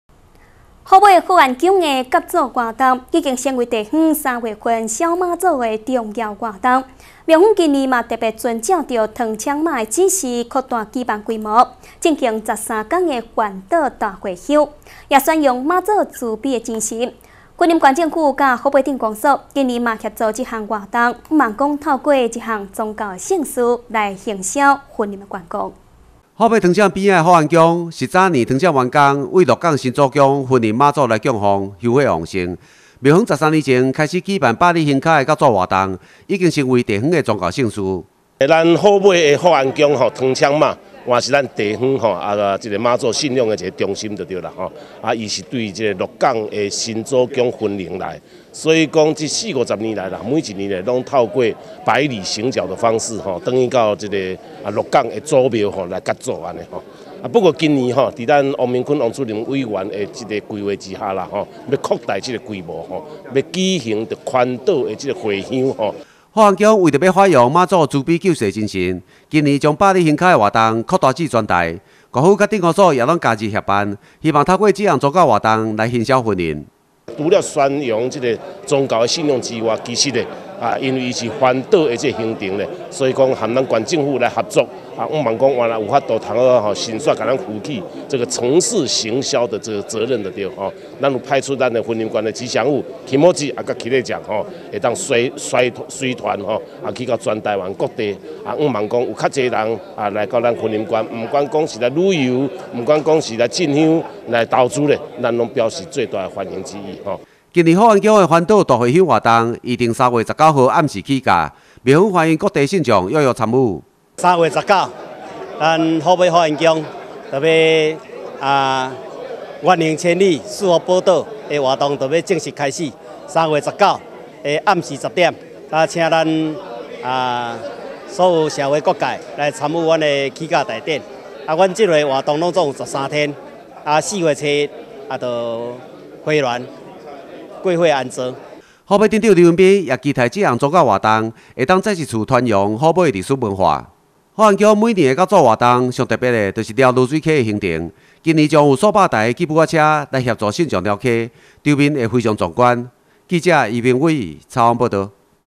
雲林新聞網─虎尾福安宮願行千里環島賜福
虎尾福安宮百里行腳的謁祖活動，已經成為地方三月「瘋媽祖」的重要活動，廟方今年特別遵照糖廠媽指示，擴大舉辦的規模，預定進行十三天的環島大會香活動，宣揚媽祖慈悲濟世的精神，雲林縣政府與虎尾鎮公所今年也協助這項活動，期待透過這項宗教盛事，來行銷雲林的宗教與觀光。〈以上新聞可從top NEWS APP收視〉